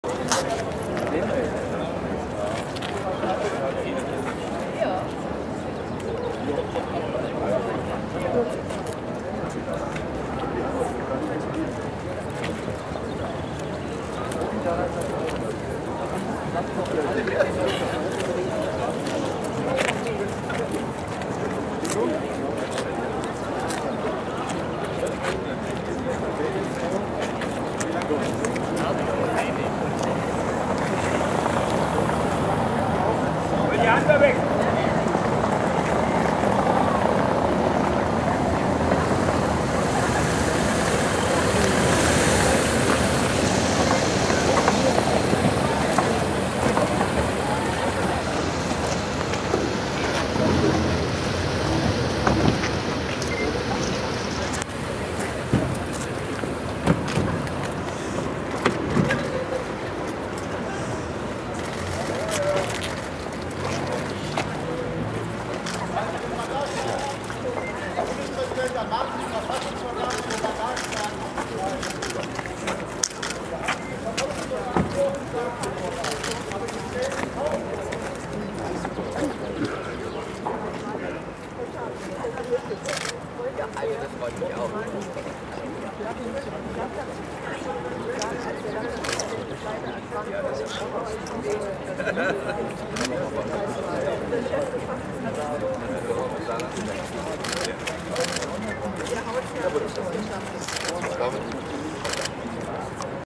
Ankunft des Konvois des Bundespräsidenten in Saarbrücken
Am 23. April 2013 machte der Bundespräsident seinen Antrittsbesuch im Saarland. Wir waren dazu eingeladen, da meine Verlobte mit unserer Tochter eingebürgert wurde. Hier dazu das ein oder andere Video mal aus einer anderen Perspektive :-) Etwas verwackelt am Anfang, aber sorry... Wenn man nicht alles selbst macht ;-) War aber auch viel los da draussen.